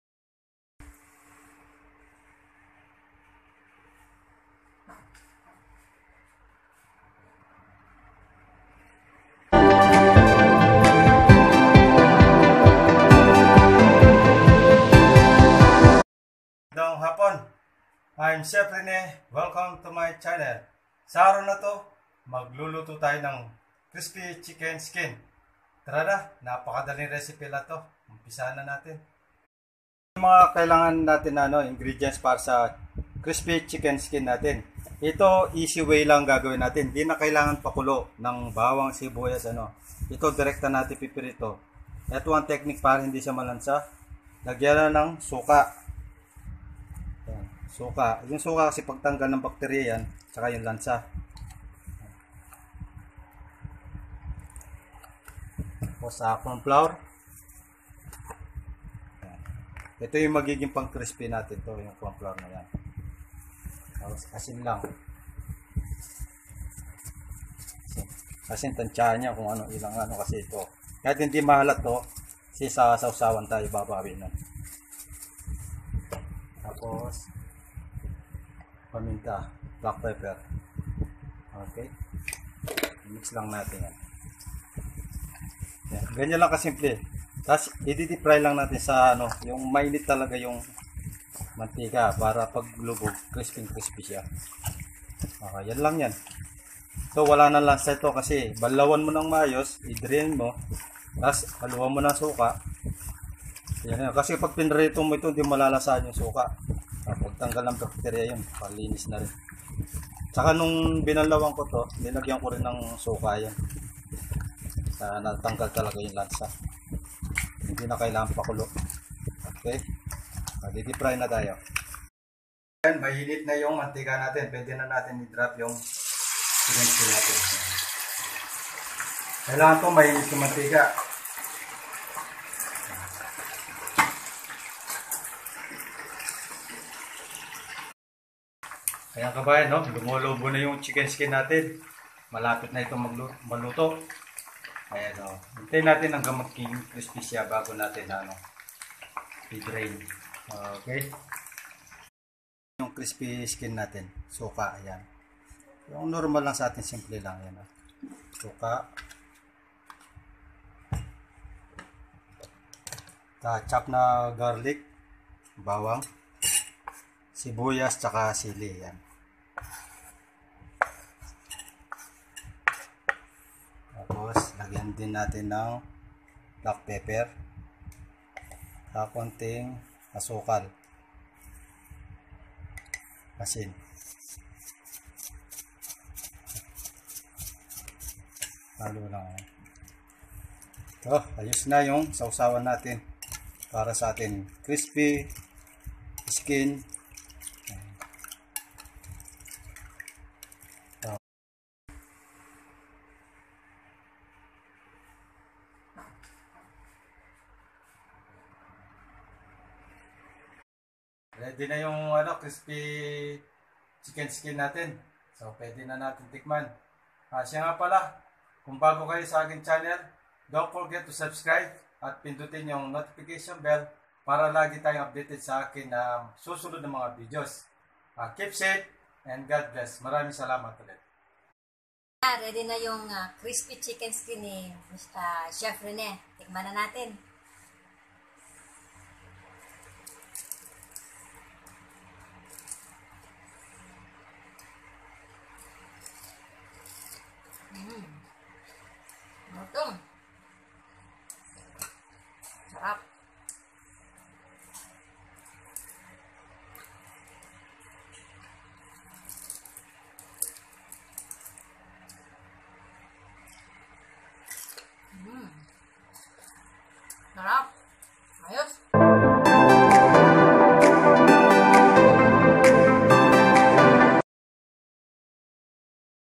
Crispy Chicken Skin ala Chef Rene
Enjoy this delicious and crispy chicken skin in easy way. Marinate the chicken skin with vinegar, salt, pepper powder, corn flour then dip fried.